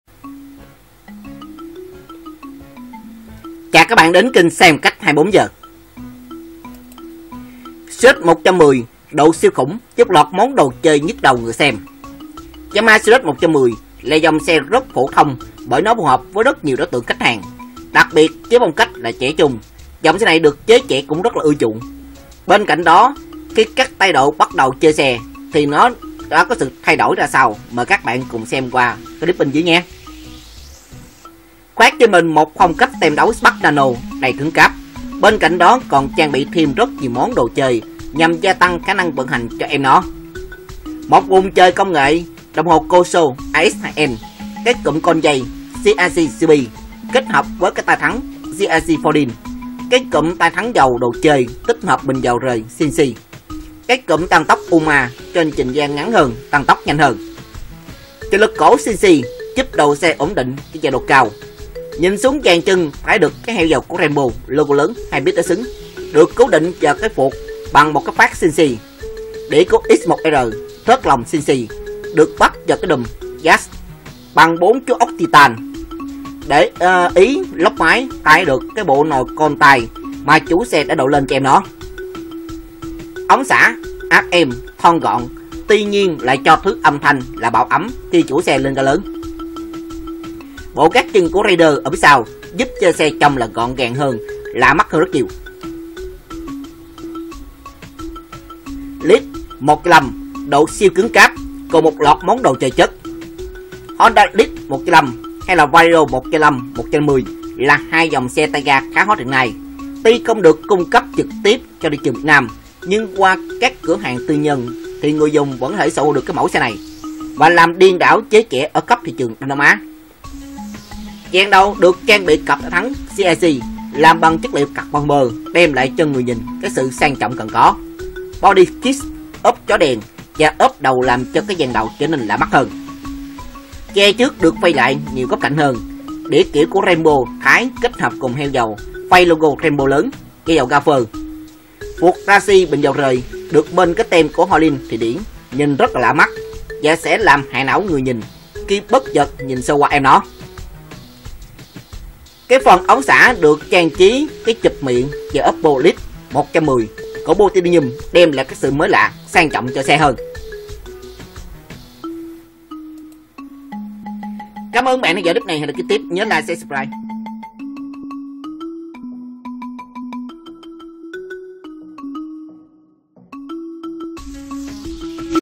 Xe & Phong Cách 24h - Sirius 110 độ siêu khủng với loạt đồ chơi nhức đầu người xem.
Yamaha Sirius 110 là dòng xe khá phổ thông bởi nó phù hợp với nhiều đối tượng khách hàng, đặc biệt với phong cách trẻ trung, dòng xe này được giới trẻ khá ưa chuộng. Bên cạnh đó thi đến tay các dân chơi xe độ thì nó sẽ được thay đổi ra sao mời các bạn xem qua loạt ảnh bên dưới.